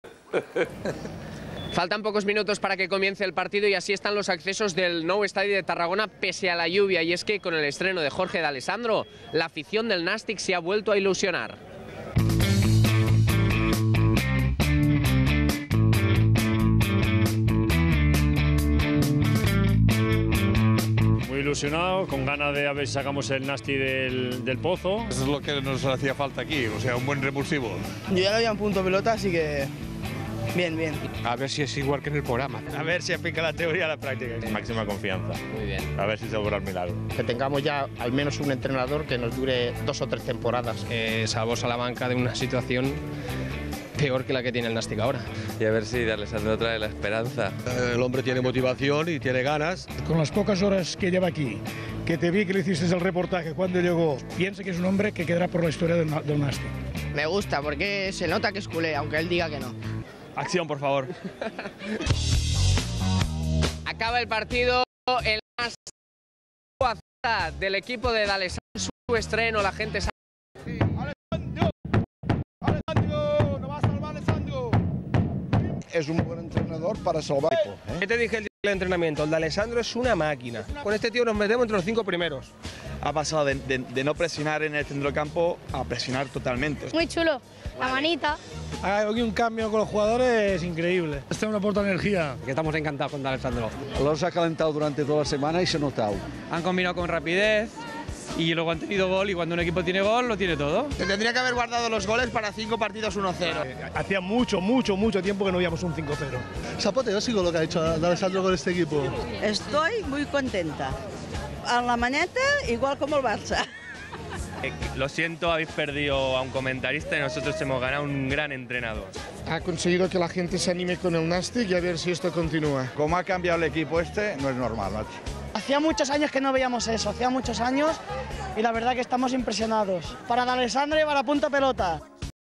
Así fue el debut de D'Alessandro
Quim Doménech recogió la opinión de los aficionados y las sensaciones de los aficionados del Nástic de Tarragona tras la espectacular victoria del equipo ante el Sabadell, en el debut de Jorge D'Alessandro en el banquillo.